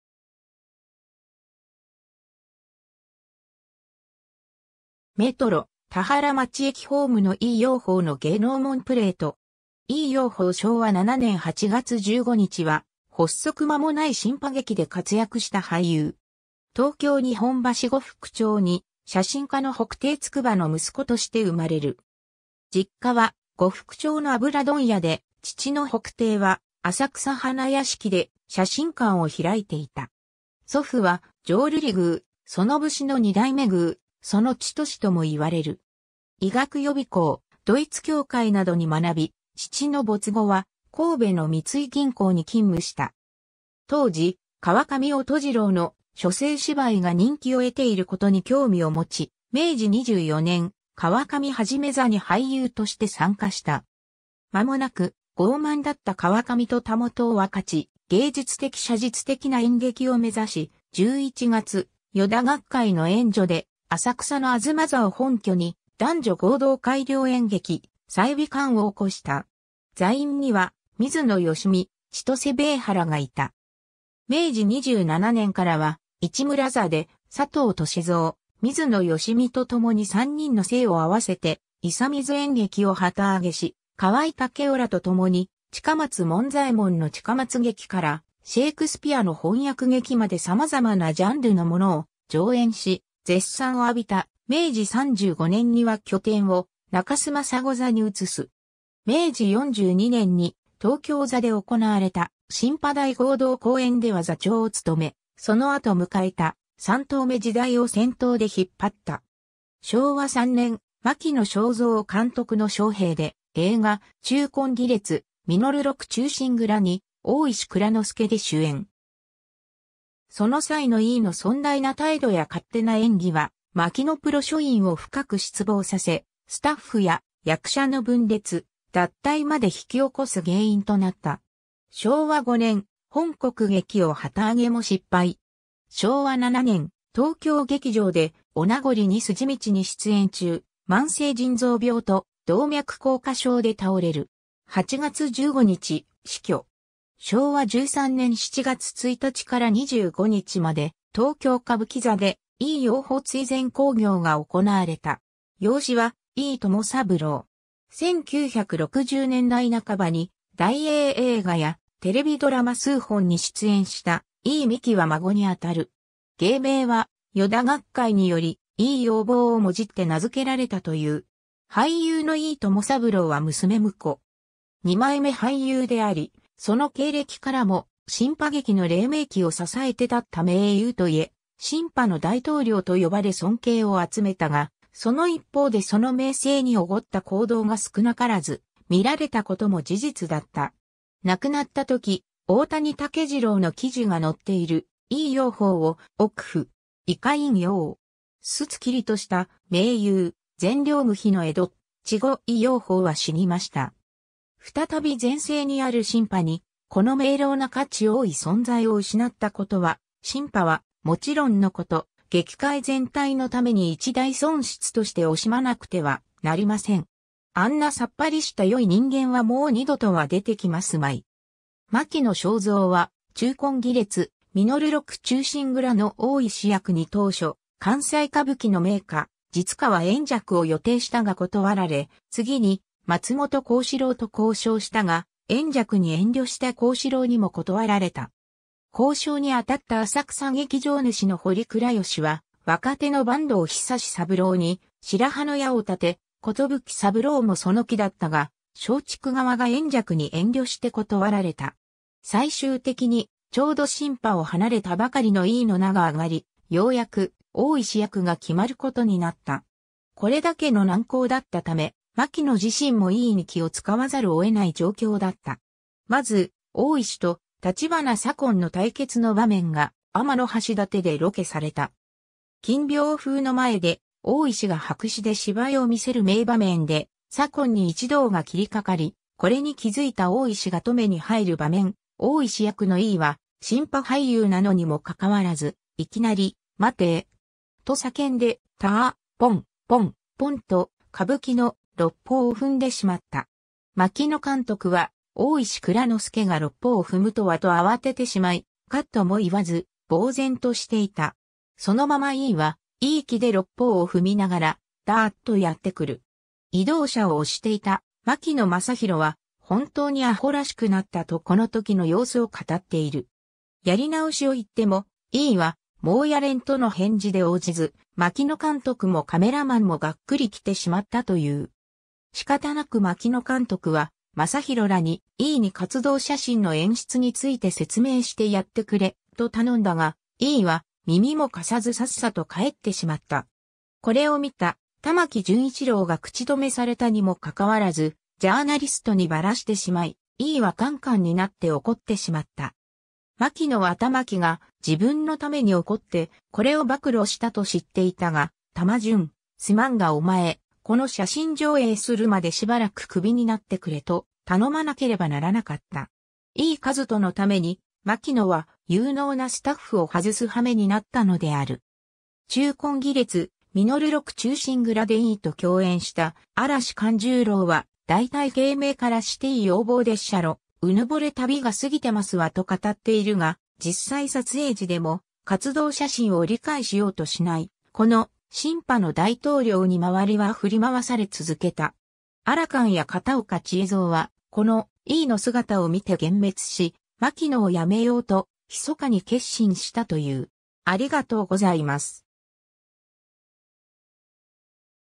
伊井蓉峰
メトロ田原町駅ホームの伊井蓉峰の芸能紋プレート。
 伊井 蓉峰（いい ようほう、本名・伊井 申三郎、明治4年8月16日（1871年9月30日） - 昭和7年（1932年）8月15日）は、発足間もない新派劇で活躍した俳優。
 東京日本橋呉服町（現・中央区）に写真家の北庭筑波の息子として生れる。
実家は呉服町の油問屋で、父の北庭は浅草花屋敷で写真館を開いていた。
祖父は浄瑠璃宮薗節の2代目宮薗千寿ともいわれる。
医学予備校、独逸協会などに学び、父の没後は神戸の三井銀行に勤務した。
 当時、川上音二郎の「書生芝居」が人気を得ていることに興味を持ち、明治24年（1891年）、「川上一座」に俳優として参加した。
間もなく、傲慢だった川上と袂を分かち、芸術的写実的な演劇を目指し、11月、依田学海の援助で浅草の吾妻座を本拠に男女合同改良演劇「済美館」を興した。
座員には水野好美、千歳米坡らがいた。
 明治27年（1894年）からは市村座で佐藤歳三、水野好美とともに3人の姓を合わせて「伊佐水演劇」を旗揚げし河合武雄らとともに近松門左衛門の近松劇からシェイクスピアの翻訳劇まで様々なジャンルのものを上演し、絶賛を浴びた、明治35年（1902年）には拠点を中洲真砂座に移す。
 明治42年（1909年）に東京座で行われた「新派大合同公演」では座長を務め、その後迎えた「三頭目時代」を先頭で引っ張った。
 昭和3年（1928年）、マキノ省三監督の招聘で映画『忠魂義烈・實録忠臣蔵』に大石内蔵之助で主演。
その際の伊井の尊大な態度や勝手な演技はマキノプロ所員を深く失望させ、スタッフや役者の分裂・脱退まで引き起こす原因となった。
 昭和5年（1930年）、本国劇を旗揚げも失敗。
 昭和7年（1932年）、東京劇場で「お名残二筋道」に出演中、慢性腎臓病と動脈硬化症で倒れる。
8月15日、死去。
 昭和13年（1938年）7月1日から25日まで　東京歌舞伎座で伊井蓉峰追善興行が行われた。
 養子は伊井友三郎。
1960年代半ばに大映映画やテレビドラマ数本に出演した伊井美貴は孫にあたる。
 芸名は依田学海により「いい容貌」をもじって名付けられたという。
俳優の伊井友三郎は娘婿。
二枚目俳優であり、その経歴からも新派劇の黎明期を支えて立った名優と言え、「新派の大統領」と呼ばれ尊敬を集めたが、 その一方でその名声に驕った行動が少なからず見られた事も事実だった。
亡くなった時、大谷竹次郎の記事が載っている「伊井蓉峰を憶ふ」。
以下引用。
 『すつきりとした名優、善良無比の江戸っ兒伊井蓉峰は死にました。
再び全盛にある新派にこの明朗な価値多い存在を失った事は新派は勿論の事、劇界全體の為に一大損失として惜しまなくてはなりません。
あんなさつぱりした好い人間はもう二度とは出て来ますまい。
（以下略）』 マキノ省三は『忠魂義烈・實録忠臣蔵』の大石役に当初、関西歌舞伎の名家實川延若 (2代目)を予定したが断られ、次に松本幸四郎 (7代目)と交渉したが、延若に遠慮した幸四郎にも断られた。
交渉にあたった浅草劇場主の堀倉吉は若手の阪東壽三郎 (3代目)に白羽の矢を立て、壽三郎もその気だったが松竹側が延若に遠慮して断られた。
最終的にちょうど新派を離れたばかりの伊井の名が上がり、ようやく大石役が決まることになった。
これだけの難航だったため、マキノ自身も伊井に気を使わざるを得ない状況だった。
 まず、大石と立花左近の対決の場面が天橋立でロケされた。
金屏風の前で大石が白紙で芝居を見せる名場面で、左近に一同が斬りかかり、これに気づいた大石が止めに入る場面、大石役の伊井は新派俳優なのにもかかわらず、いきなり「待てェ!」と叫んで「ターッ、ポン、ポン、ポン」と歌舞伎の「六方」を踏んでしまった。
マキノ監督は「大石内蔵助が六方を踏むとは」と慌ててしまい、「カット」も言わず、呆然としていた。
そのまま伊井はいい気で六方を踏みながらダーッとやって来る。
移動車を押していたマキノ雅弘は「本当にアホらしくなった」とこのときの様子を語っている。
やり直しを言っても伊井は「もうやれん」との返事で応じず、マキノ監督もカメラマンもがっくり来てしまったという。
 仕方なくマキノ監督は雅弘らに「伊井に活動写真の演出について説明してやってくれ」と頼んだが、伊井は耳も貸さずさっさと帰ってしまった。
これをみた玉木潤一郎が口止めされたにもかかわらずジャーナリストにばらし...